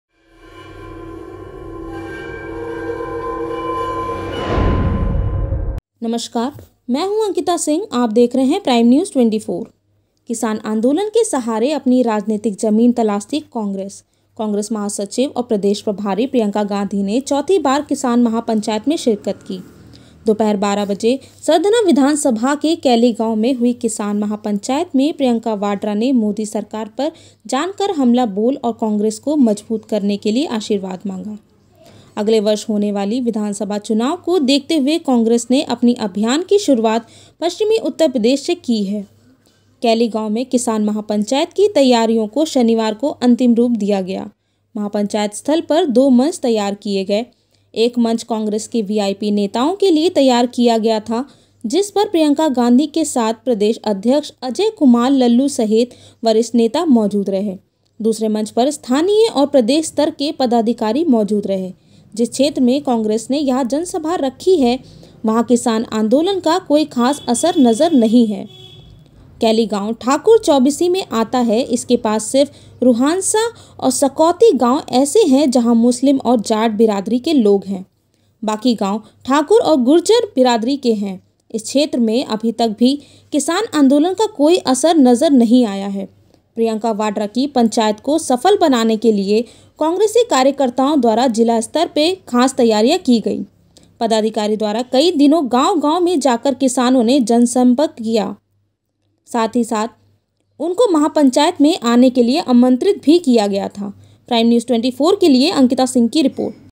सरधना विधानसभा के कैली गांव में केंद्र सरकार पर जानकर बरसी प्रियंका गांधी
कांग्रेस की राष्ट्रीय महासचिव प्रियंका वाड्रा ने संबोधित करते हुए कहा कि कृषि कानून को वापस लेने के लिए कितने भी साल लग जाएं, कांग्रेस आपके साथ खड़ी रहेगी। उन्‍होंने कहा कि इसके लिए चाहे 100 दिन लग जाए या फिर 100 साल कांग्रेस और किसान पीछे नहीं हटेंगे।जबतक है दम तब तक लडूंगीं, चाहे 100 दिन हो या 100 साल
Addressing a Kisan mahapanchayat in Meerut, Uttar Pradesh, today, Priyanka Gandhi Vadra said: "This is Meerut, the place from where the first revolt of the freedom struggle started. From here our fight for freedom started. Who was involved in that freedom struggle? Who got our nation freed? The farmers of this country did."Attacking the three new farm laws, she said: "In a similar manner, the BJP government is also exploiting the farmers. It has bought three such laws that will not let you (farmers) get your income. You better know what these three laws are. One law permits hoarding by big industrialists and billionaires. They can deposit as much quantity, for as much time, and for as much price-- everything is decided by them. This is the first law."They have said that private mandis can be opened where no tax shall be charged and tax shall be levied in government mandis. Overall if we see, it seems that the opening of private mandis is good and shall facilitate the farmers. But in reality, if tax is charged in government mandis and the same is not charged in private mandis, then slowly and gradually all the farmers will start going to private mandis. This means, slowly all the government mandis will shut down. But, there is a difference of MSP between private mandis and government mandis," she added.

In government mandis, it is fixed that farmers get minimum support price, but this is not the case with private ones, Ms Gandhi said. "After some time there will be private mandis all over, government mandis will shut down and you (farmers) will stop getting MSP.